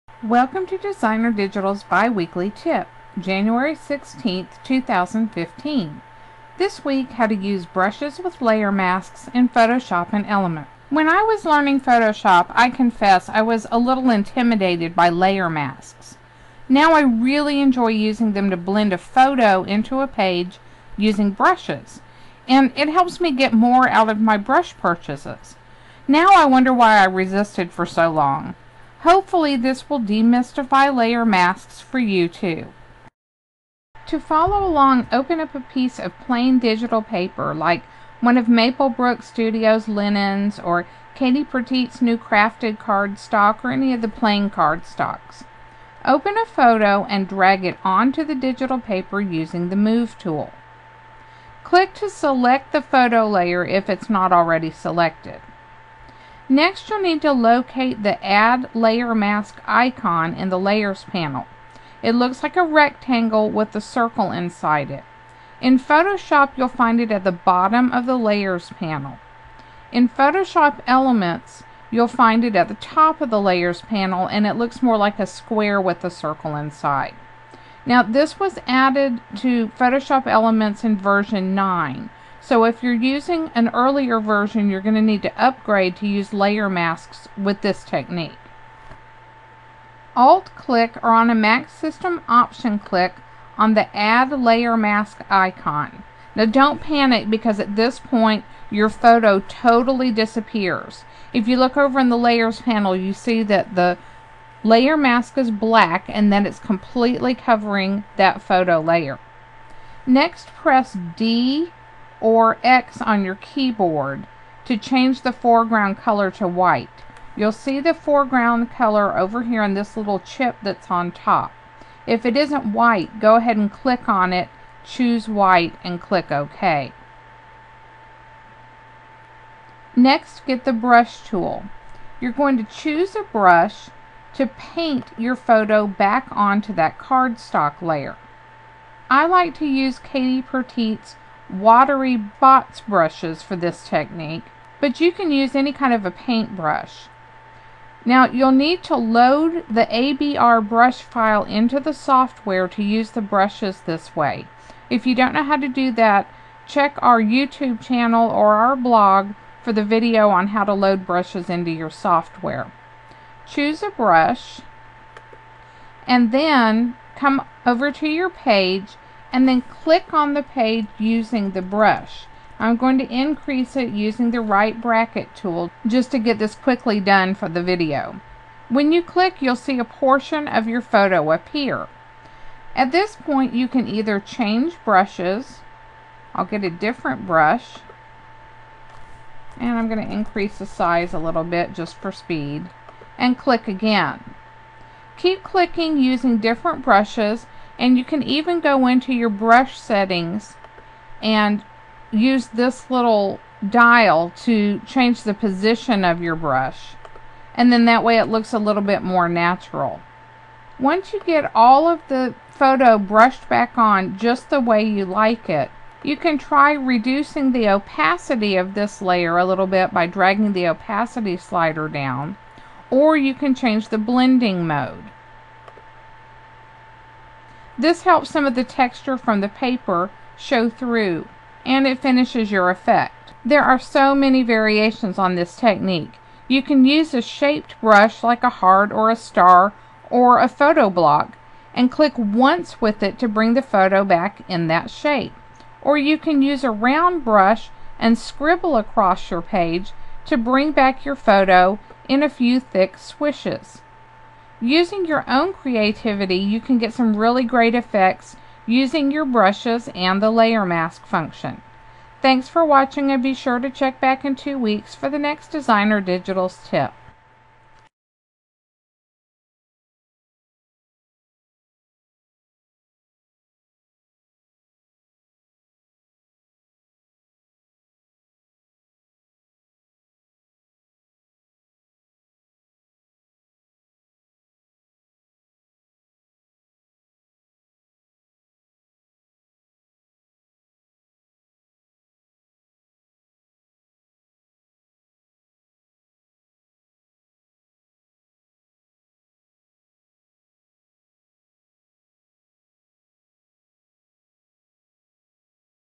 How to Use Brushes with Layer Masks in Photoshop and PSE
Adobe Photoshop and/or Photoshop Elements Video how-to from DesignerDigitals. This week: How to Use Brushes with Layer Masks in Photoshop and PSE.